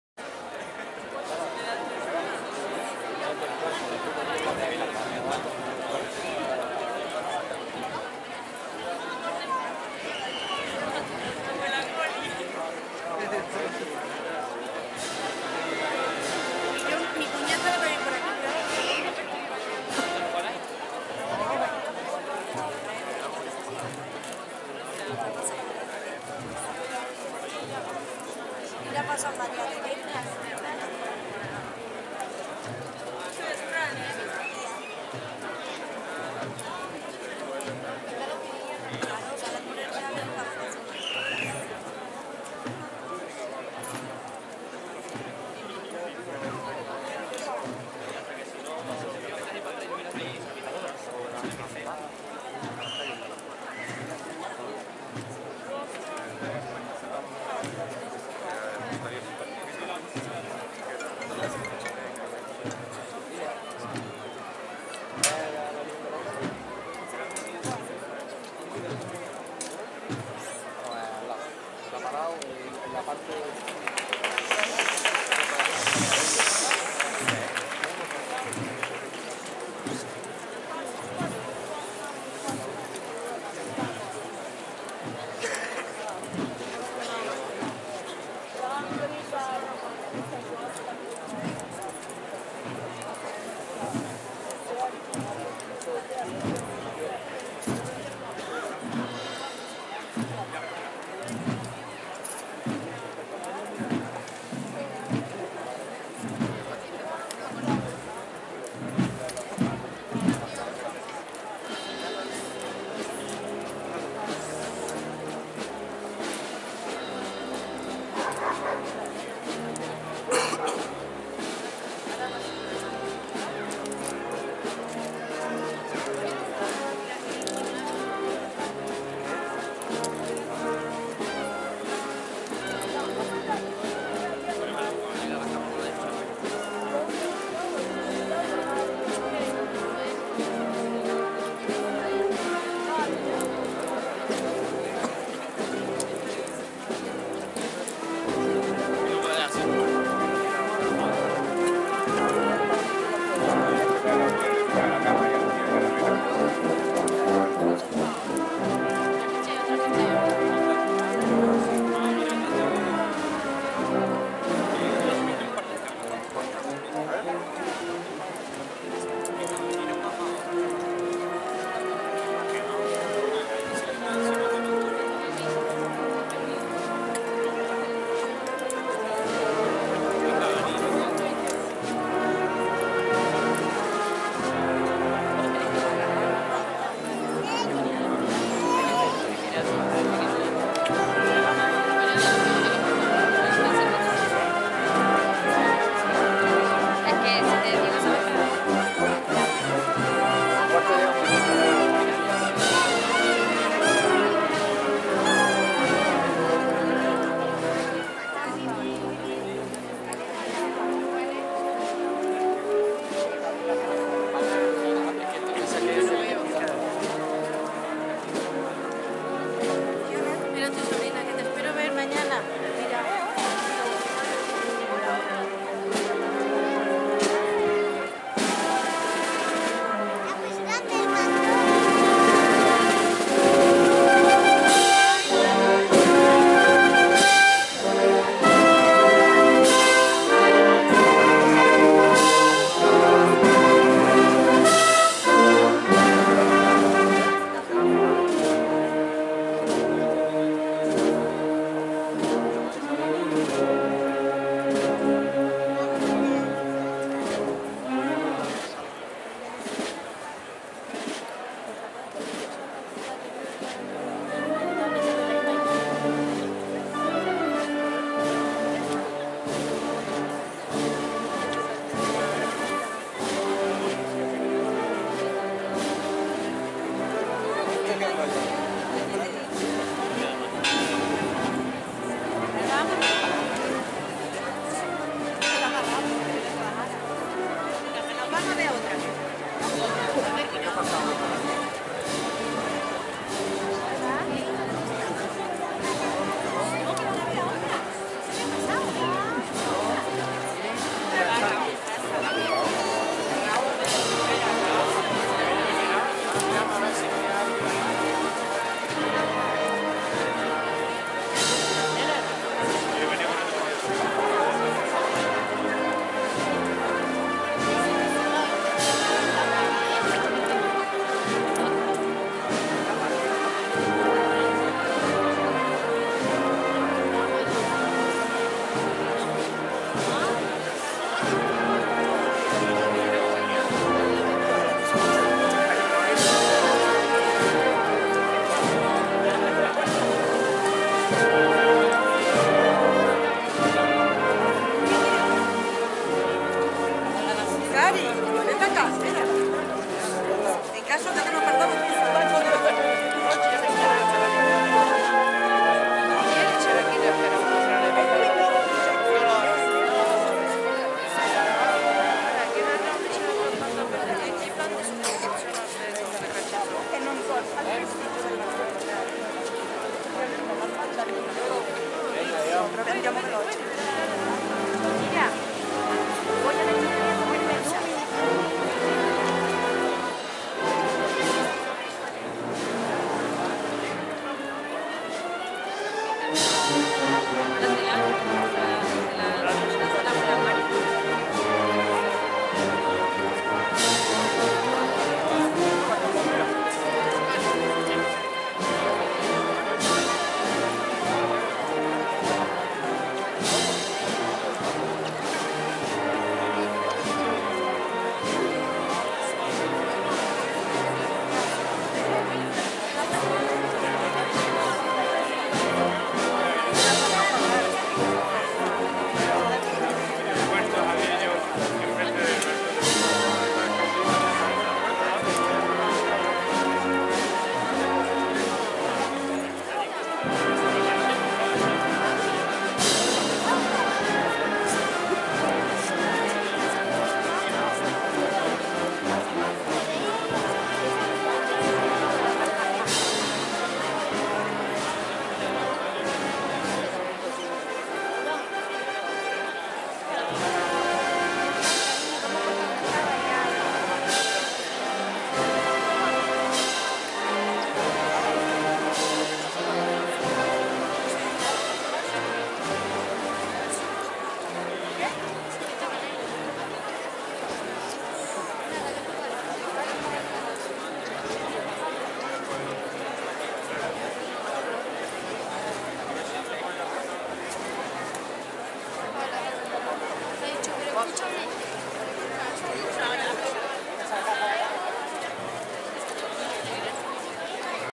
Nuestra Señora de los Reyes 2016 HD
Paso de Nuestra Señora de los Reyes por Plaza Nueva cuando se dirigía hacia la Carrera Oficial acompañada por la Banda de Música Ángeles.